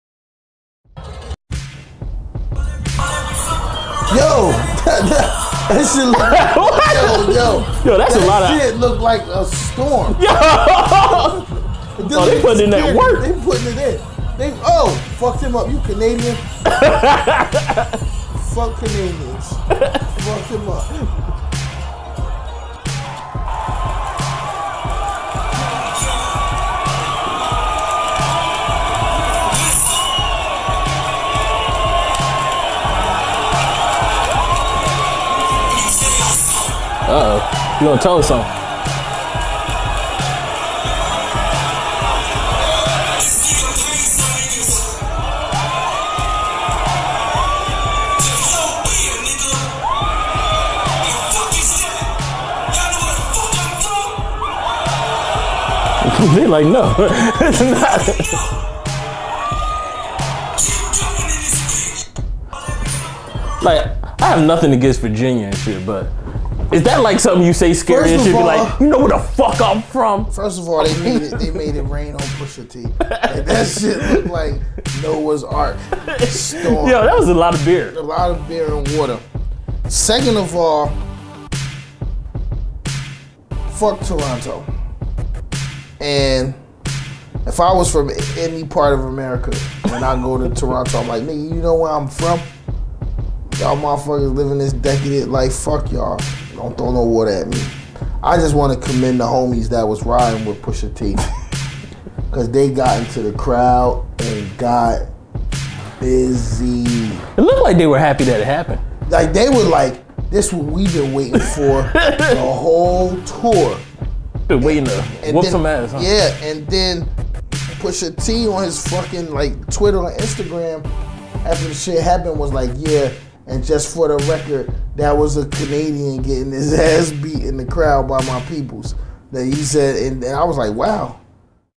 Pusha T's Toronto concert rains beer
Pusha T's Toronto concert ended super abruptly with fans throwing beer an on-stage brawl